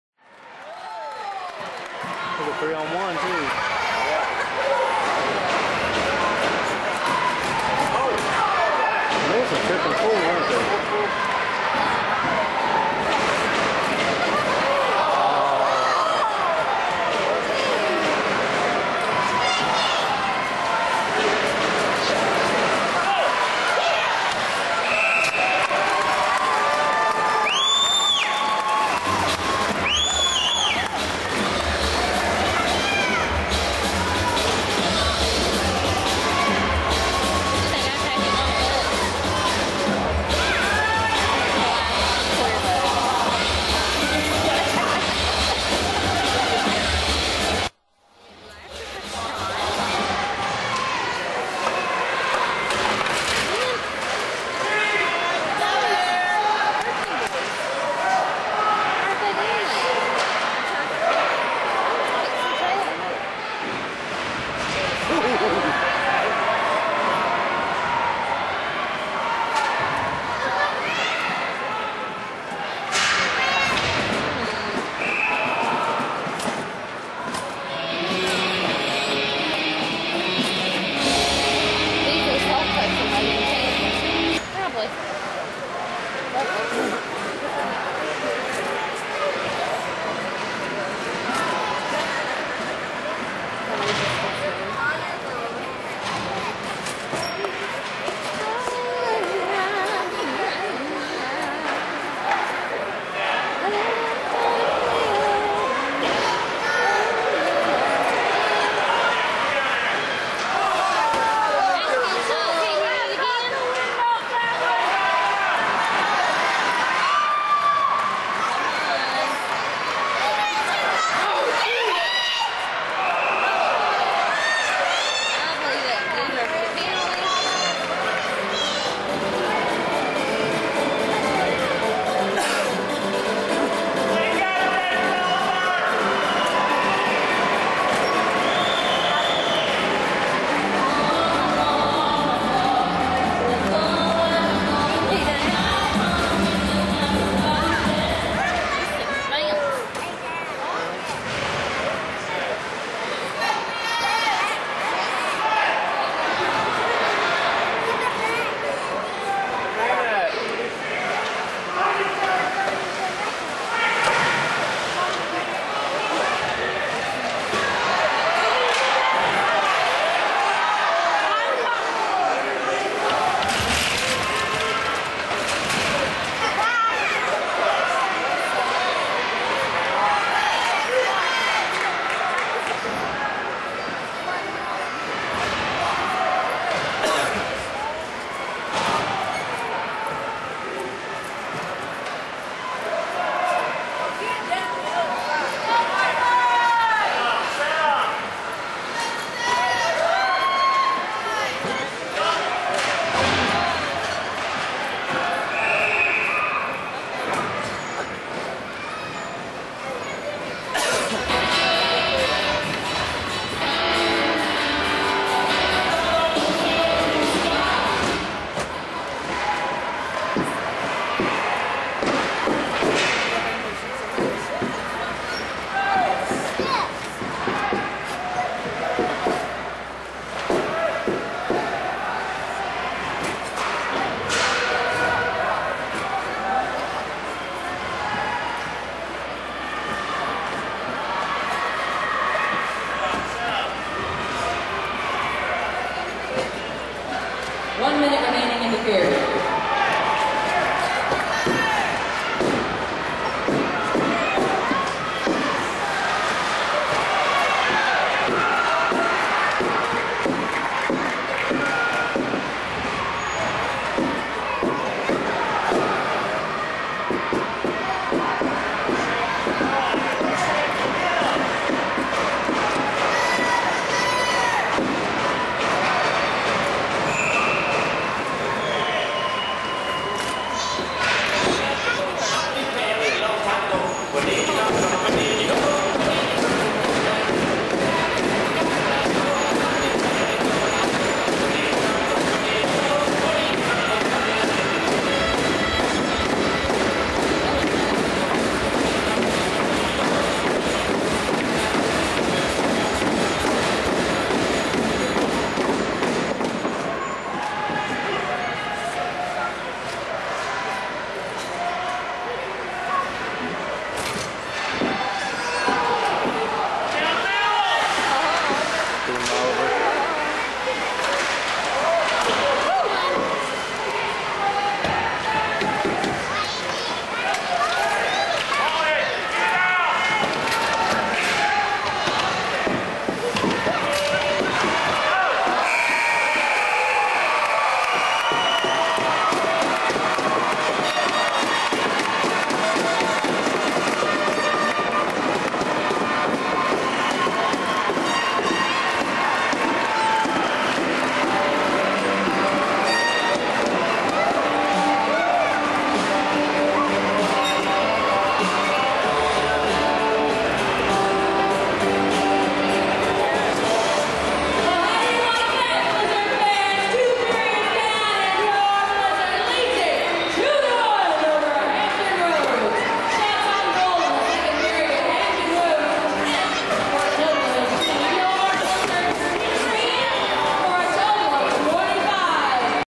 Blizzard vs Admirals 4/9/00 (8/12)
Huntington Blizzard vs Hampton Roads Admirals at the Big Sandy Superstore Arena

Taped by Mike Holley, April 9, 2000